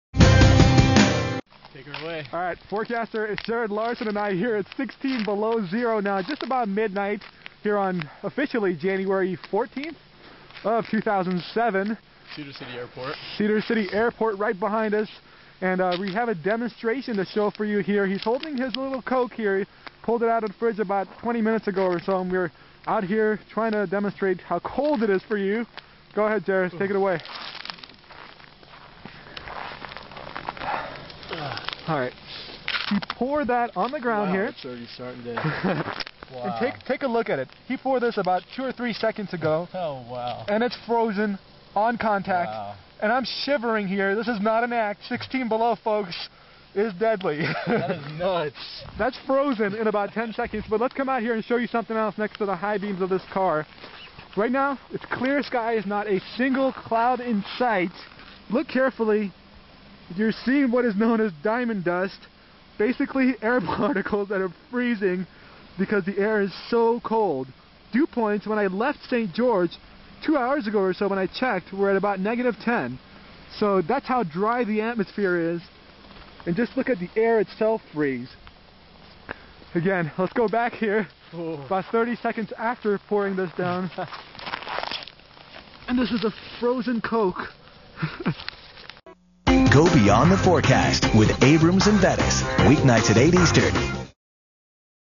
Viewer Video: Watch cola freeze instantly
P.J. Javaheri is a forecaster in St. George, UT and he and another forecaster did an experiment on 1/14/07 in 16 below zero temperatures.